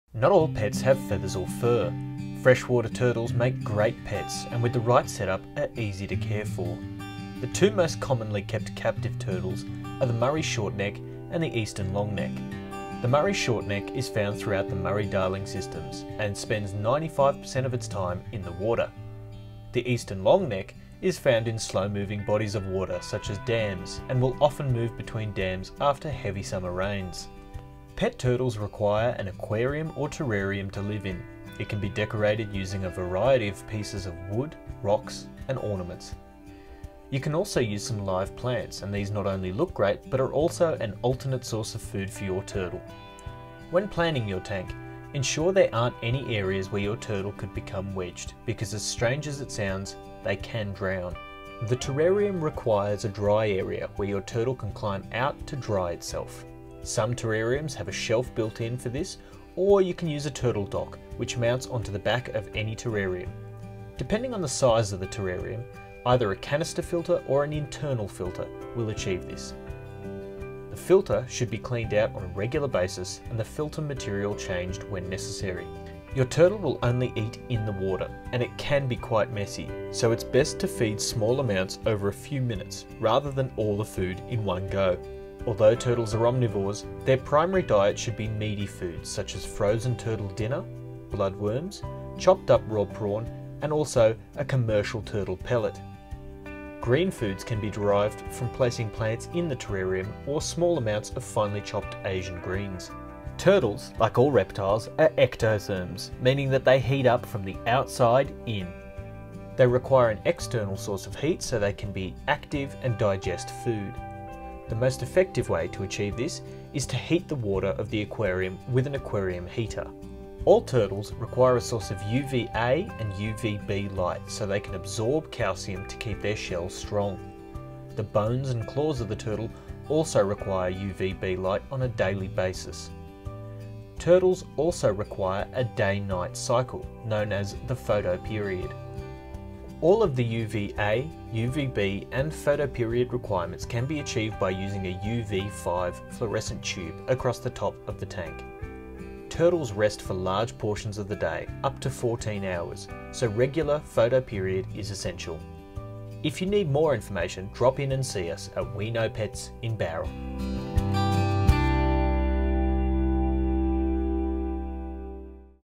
Turtles as pets Responsible pet care guide
Turtles are a pet with links to pre-historic times! In NSW you need a reptile licence to keep turtles as pets. Learn more about these fascinating creatures in our short guide.